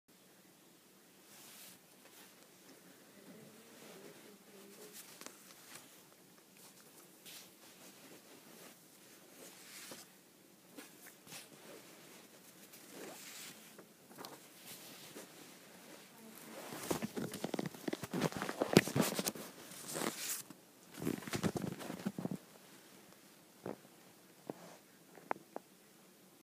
Check these awesome channels out/shoutouts!
Here I just share with you guys some YouTube channels I have done some "collabs" with, and telling you to check them out.😹